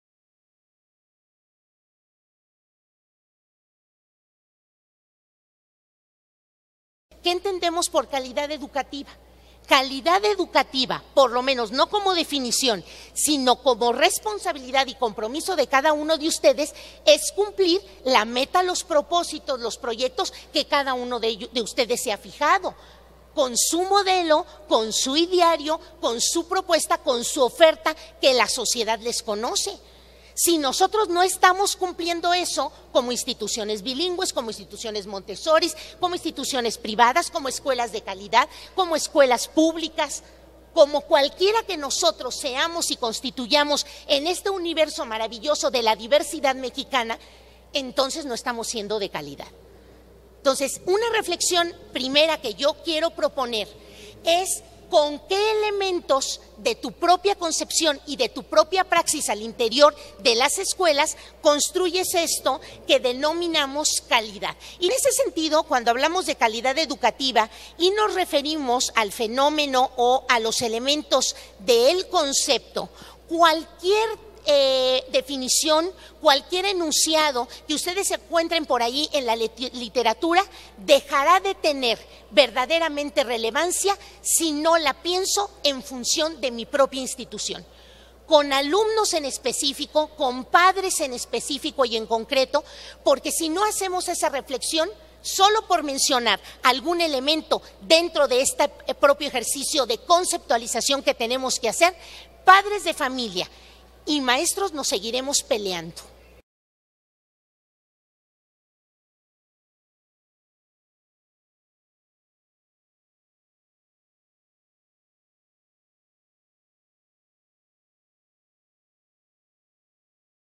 La Calidad Educativa y su conceptualización
Participación de la Dra. Patricia Ganem Alarcón sobre La Calidad Educativa y su conceptualización